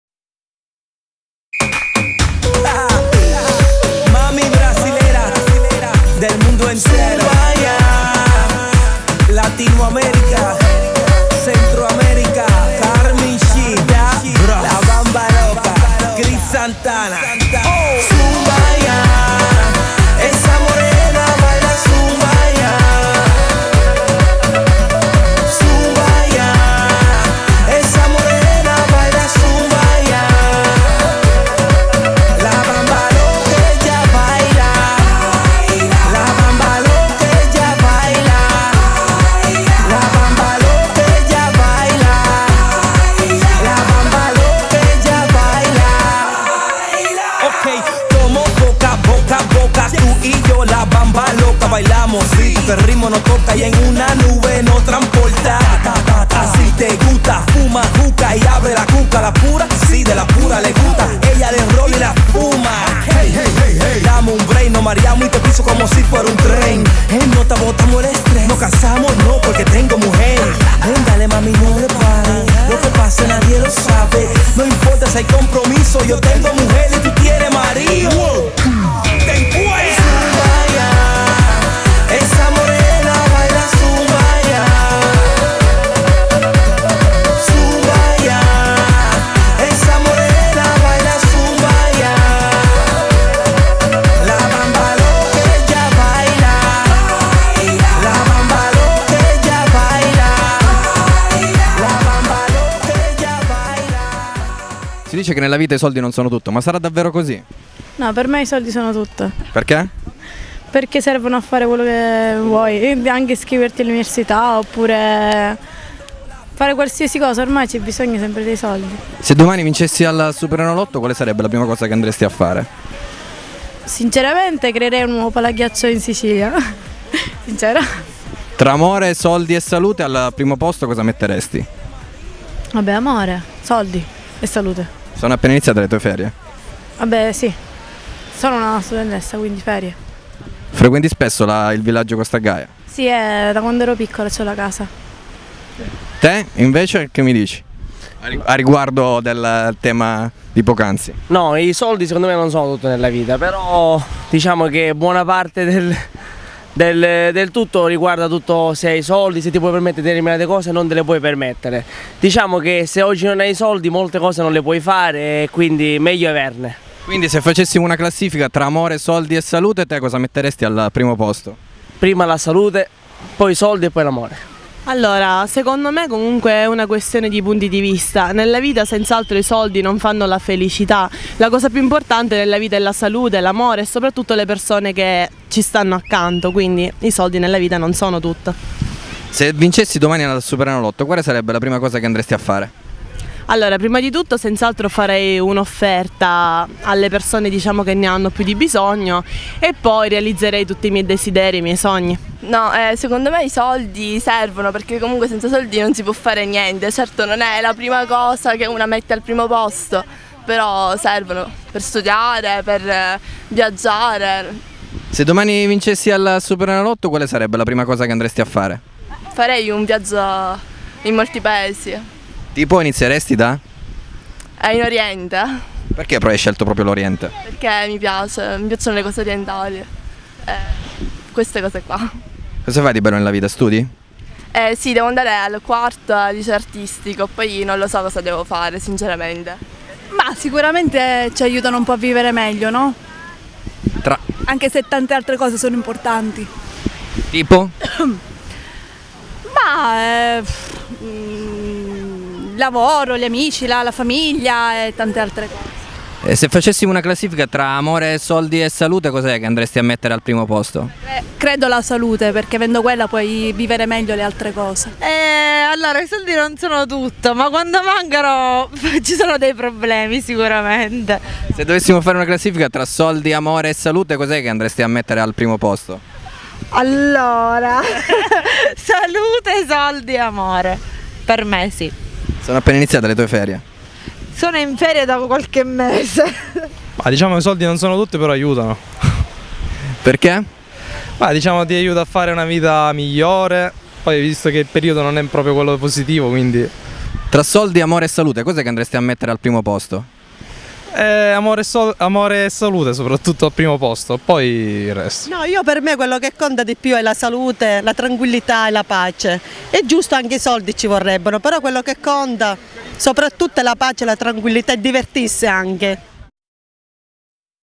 interviste nelle spiagge del litorale jonico della sicilia 26.07.2011
interviste nelle spiagge del litorale jonico della sicilia,villaggio costa gaia,villaggio marina e villaggio sabbione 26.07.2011 una produzione di ANTENNA UNO LENTINI (SR)